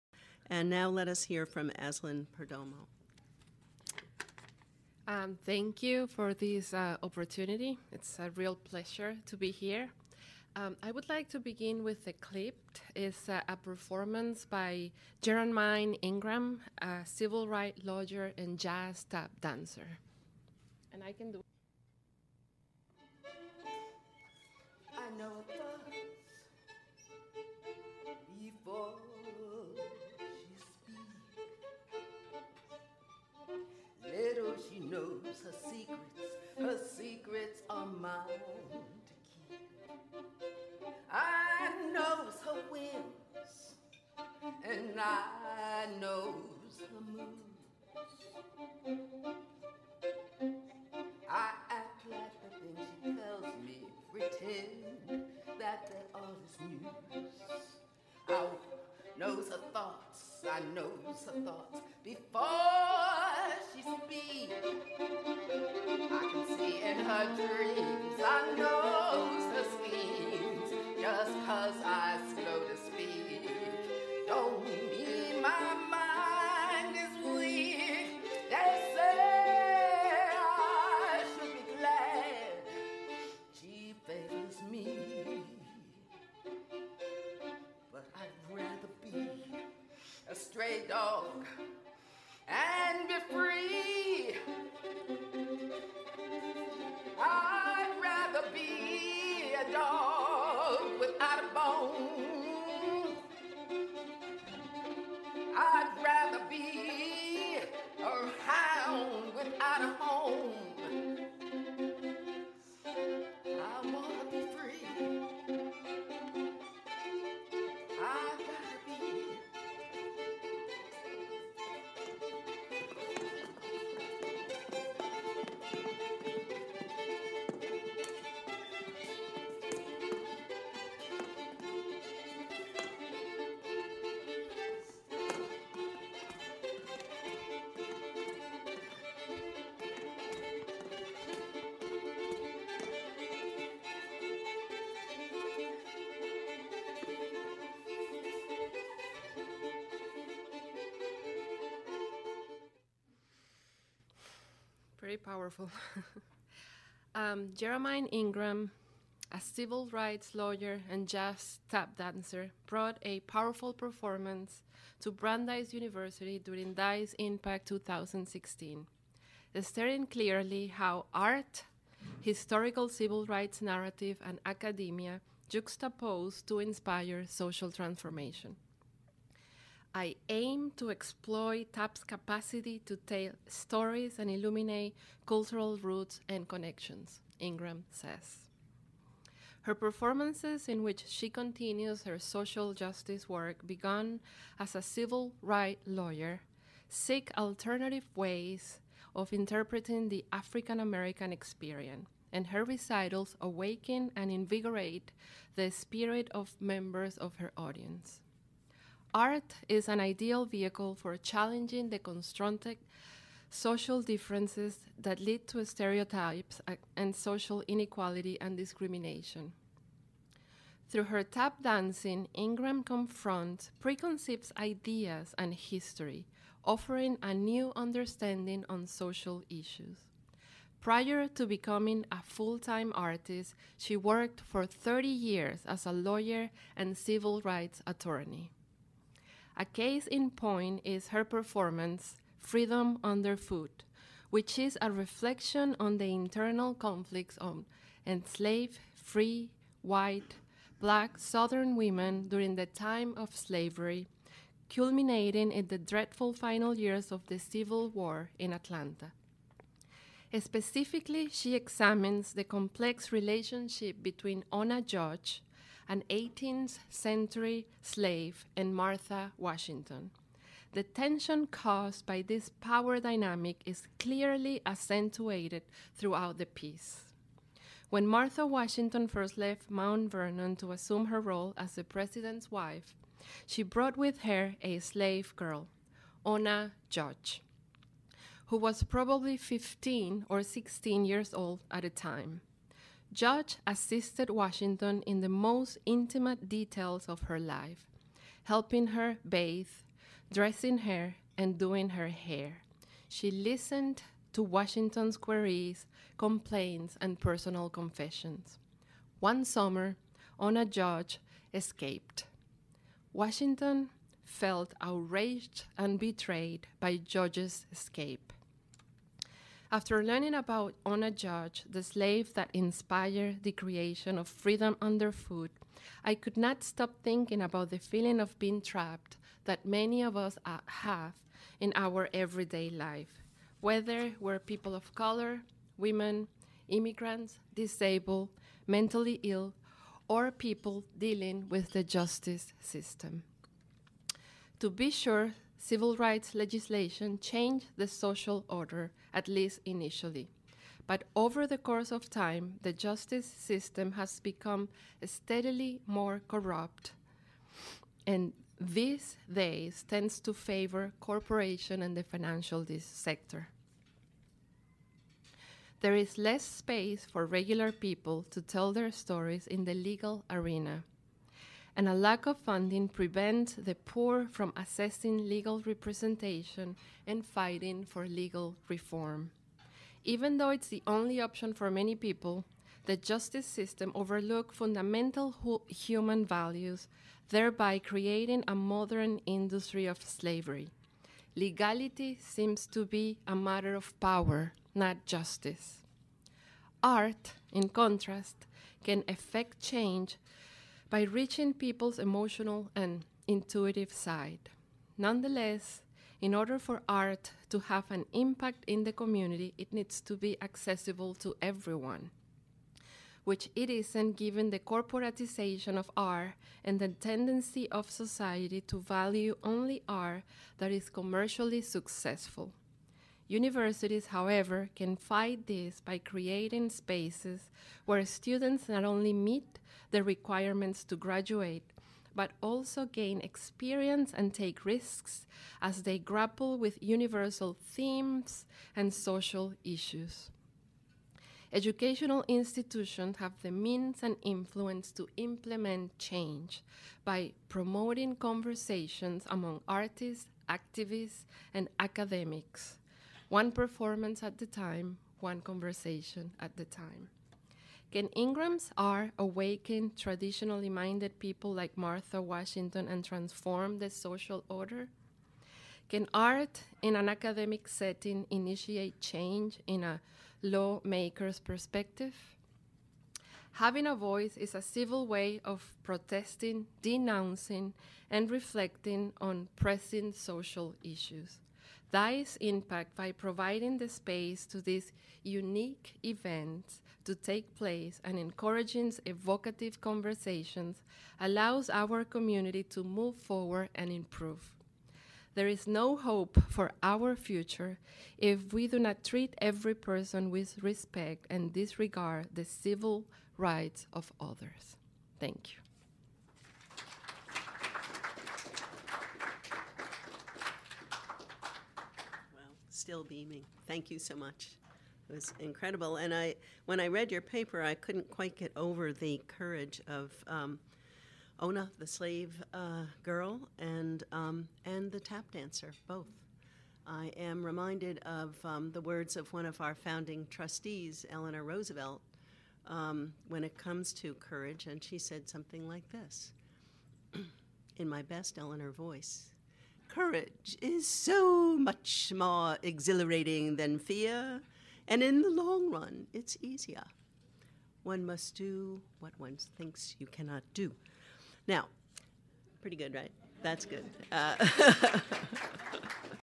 Five Years of ’DEIS Impact Reflection on 2016 Keynote- Prof. Azlin Perdomo
Revisit the first five years of ’DEIS Impact with Brandeis faculty as they reflect on themes of previous keynote speakers. This retrospective honors the work of this uniquely Brandeisian
festival of social justice and identifies opportunities for growth in the future of ‘DEIS Impact.

go.brandeis.edu/deisimpact